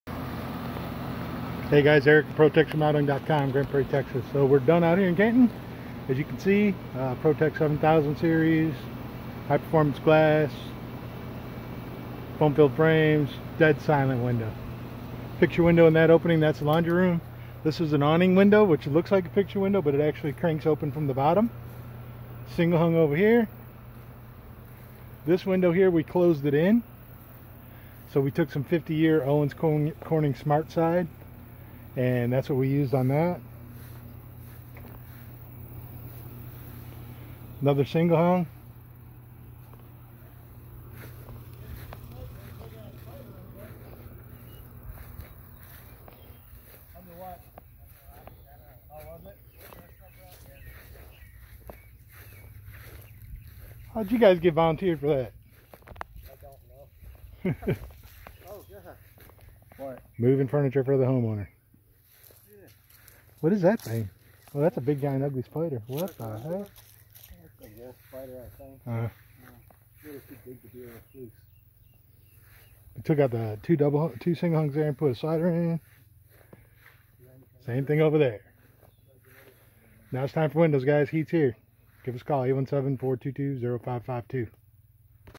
Canton Texas window installation complete
This is the ProTex 7000 Series Replacement Window System.